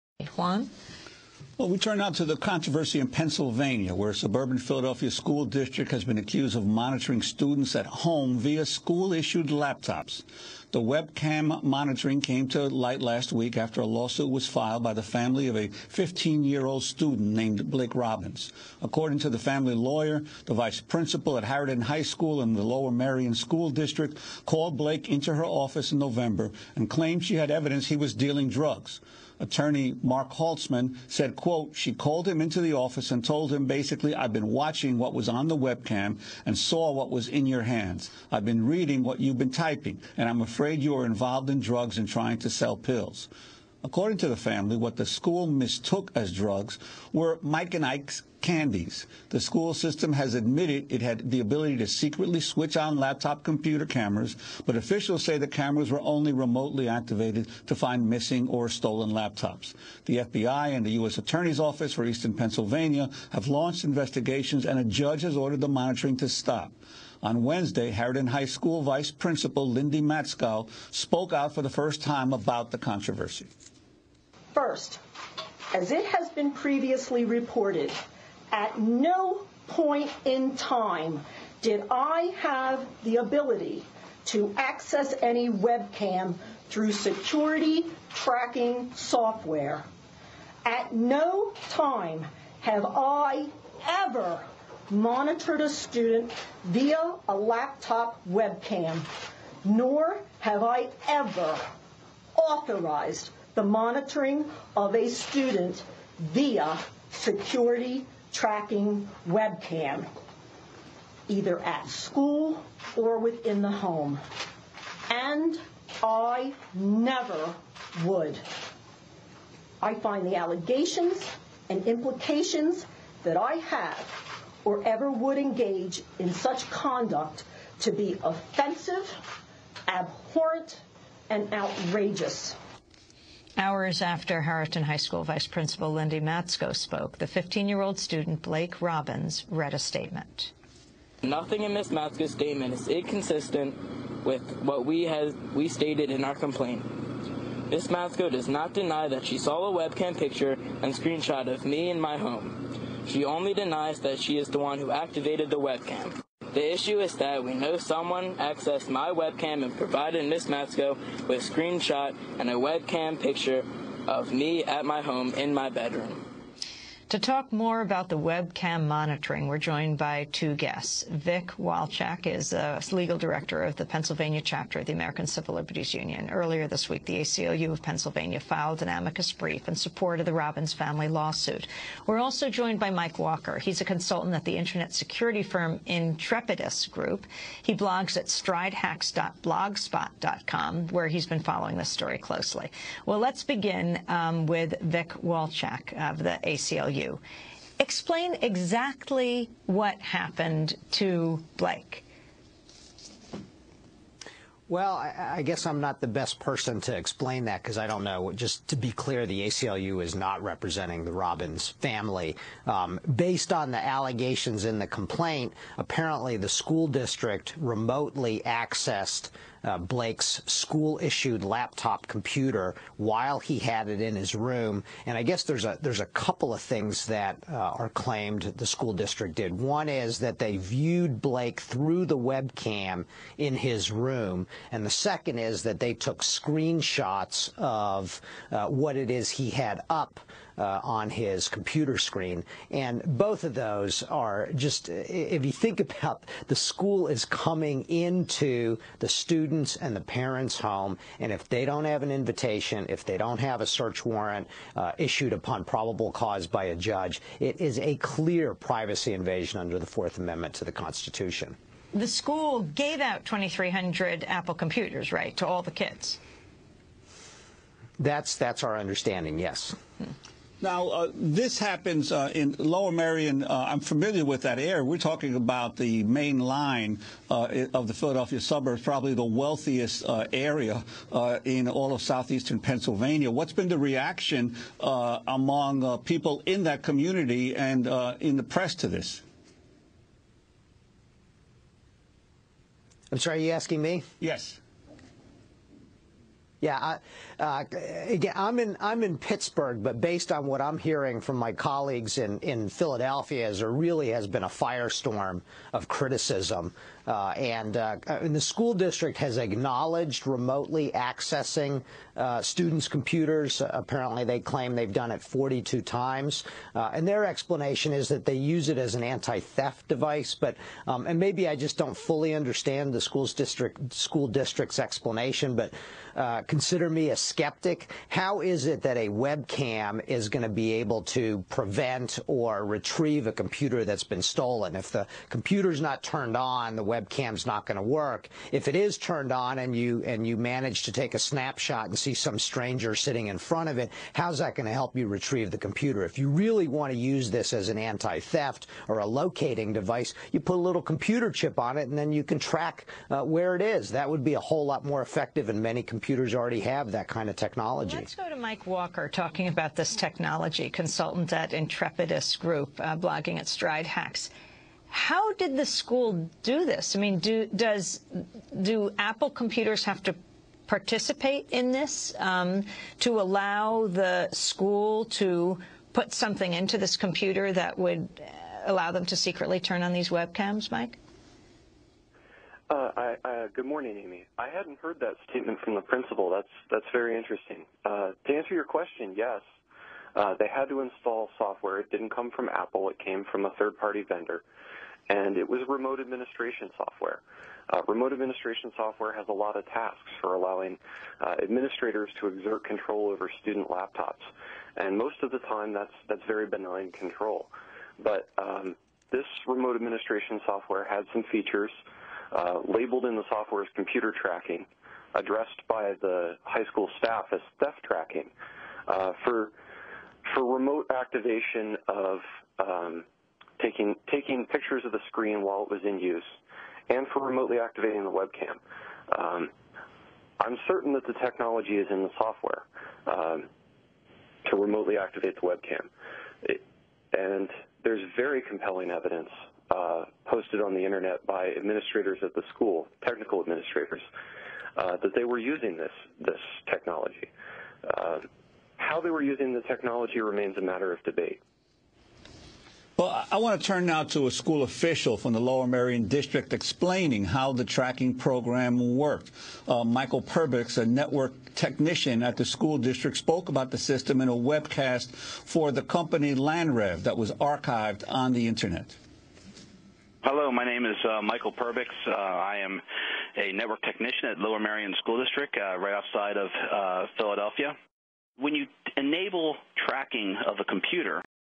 BIG BROTHER IS WATCHING YOU pt 1 School Caught Watching Students At Home from Web cam
A suburban Philadelphia school district has been caught  monitoring students at home via school-issued laptops.
The webcam monitoring came to light last week after a lawsuit was filed by the family of a 15-year-old student who had his webcam activated after the school mistook his eating of candy for the consumption of drugs. The FBI and the US Attorneys Office for eastern Pennsylvania have launched investigations and a judge has ordered the monitoring to stop.
UPDATE: A settlement for over $600,000.00 was paid by the school district upon being found guilty of invasion of privacy in a court of law.